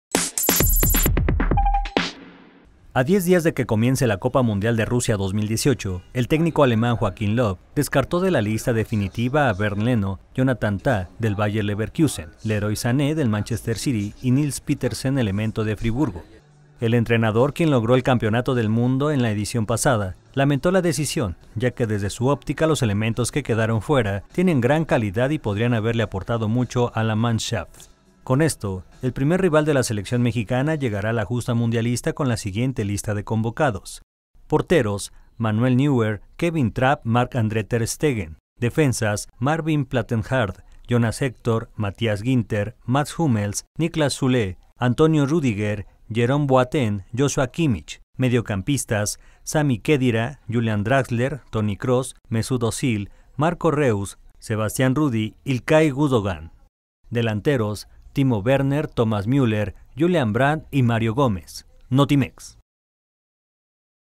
Alemania anuncia su lista oficial de convocados para Rusia 2018
A 10 días de que comience ella Copa Mundial de Rusia 2018, el técnico alemán Joachim Löw descartó de la lista definitiva a Bernd Leno, Jonathan Tah, del Bayern Levekusen, Leroy Sané, del Manchester City, y Nils Petersen, elemento del Friburgo.